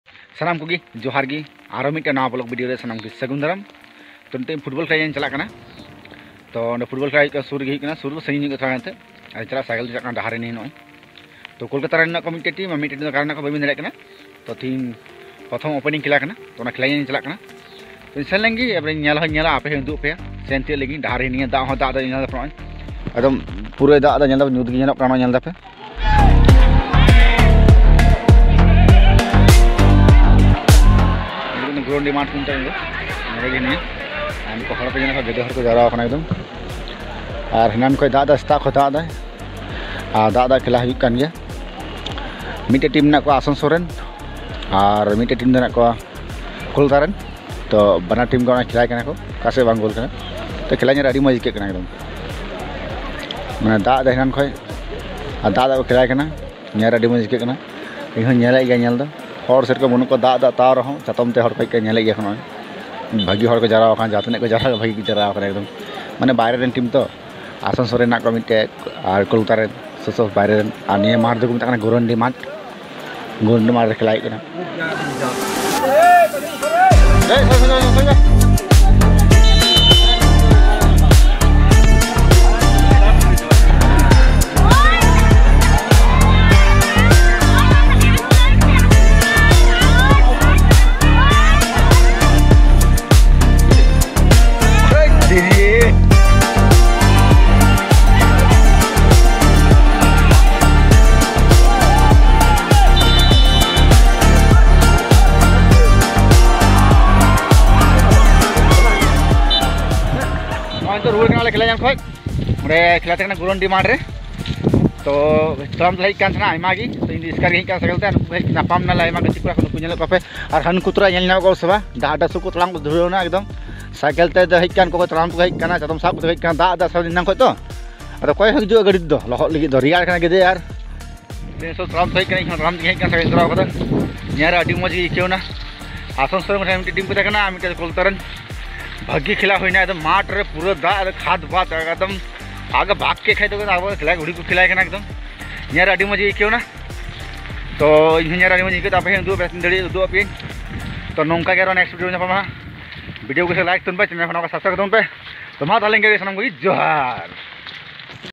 Football Opening Khela Nyaal Vlog Video 2023
Football Khela nyaal en Chala Lena
Football Khela dekhne main gaya tha